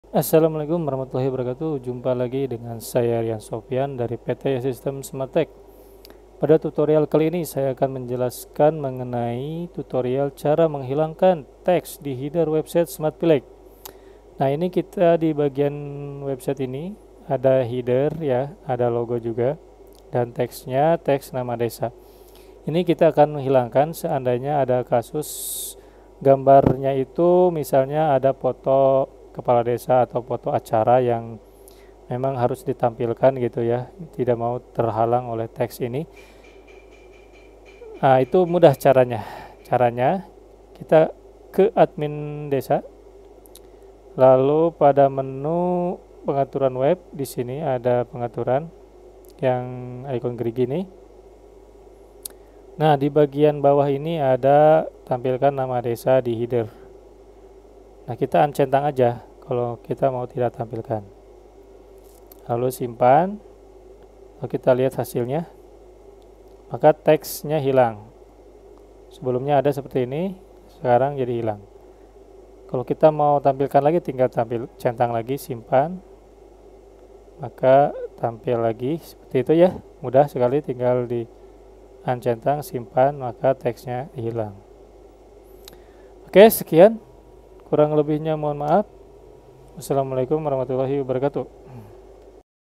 Cara Menghilangkan Teks di HEADER Website SMARTVILLAGE DESA
Tutorial Cara Menghilangkan Teks di HEADER Website SMARTVILLAGE DESA